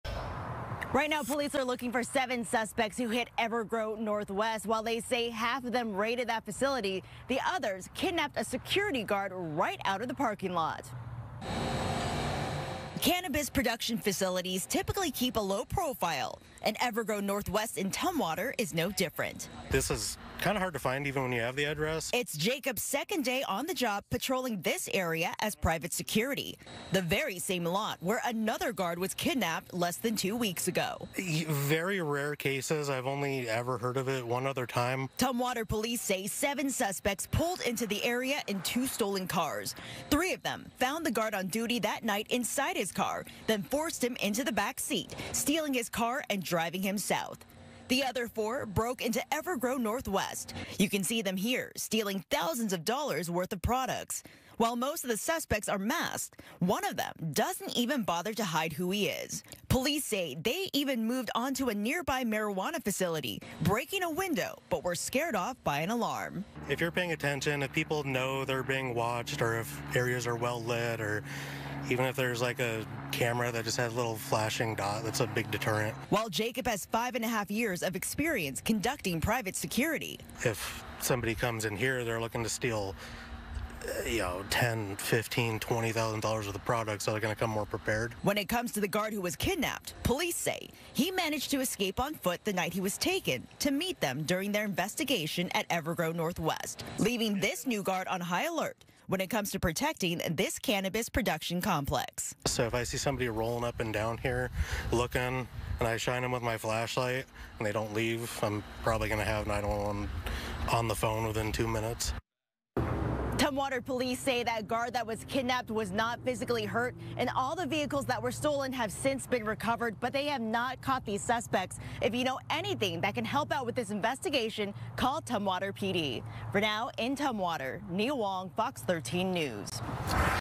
Security guard kidnapped during Tumwater marijuana facility break-in | FOX 13 Seattle
Police are working to identify multiple suspects who kidnapped a security guard in order to break into a marijuana production facility in Tumwater earlier this month.